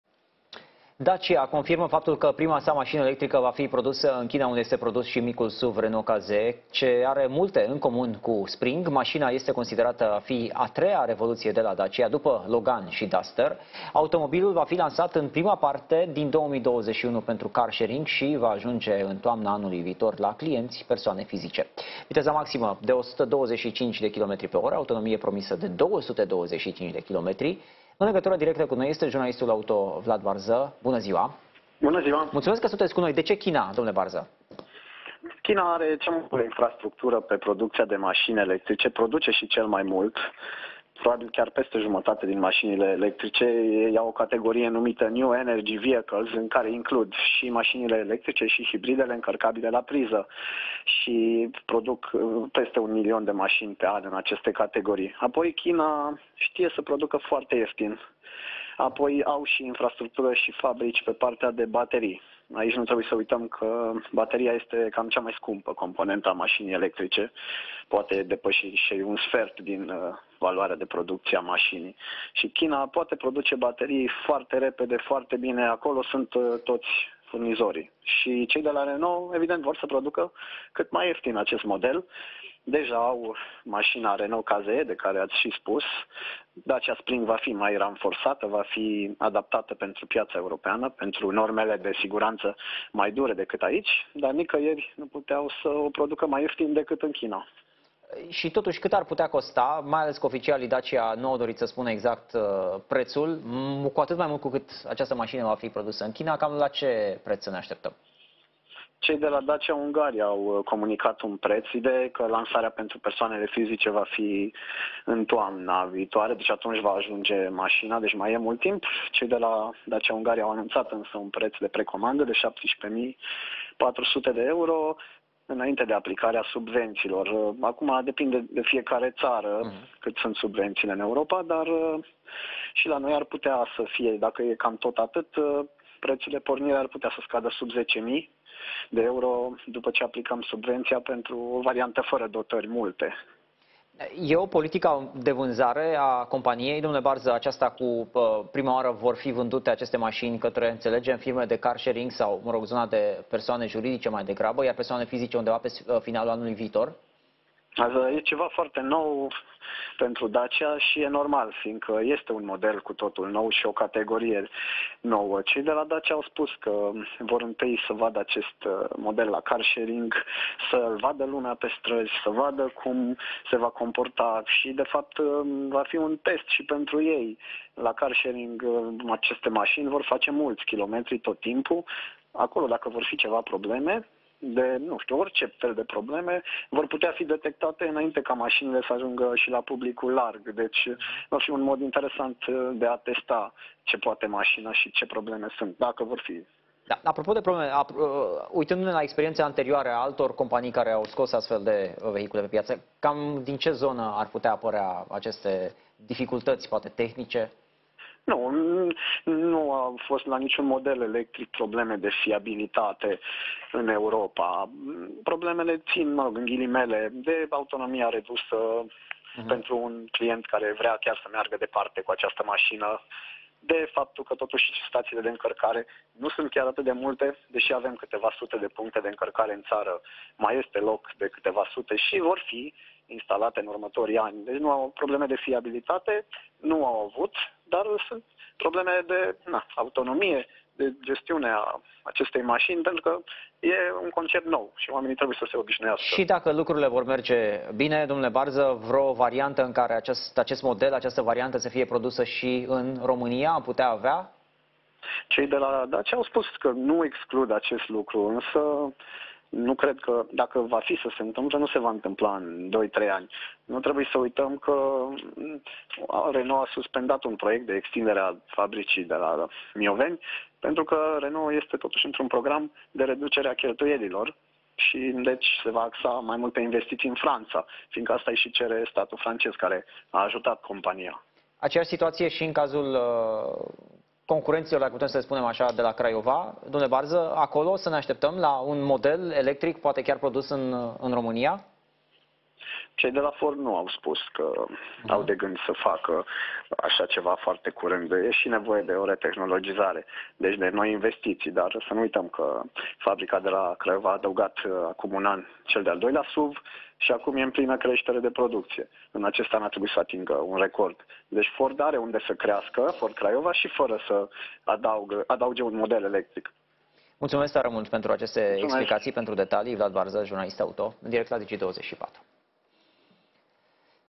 Dacia electrică va fi produsă în China
Dacia confirmă faptul că prima sa mașină electrică va fi produsă în China, unde este produs și micul SUV Renault K-ZE ce are multe în comun cu Spring. Mașina este considerat a fi "a treia revoluție de la Dacia", după Logan și Duster. Automobilul va fi lansat în prima parte din 2021 pentru car-sharing și va ajunge în toamna anului viitor la clienții persoane fizice. Viteza maximă este 125 km/h, autonomie promisă este 225 km.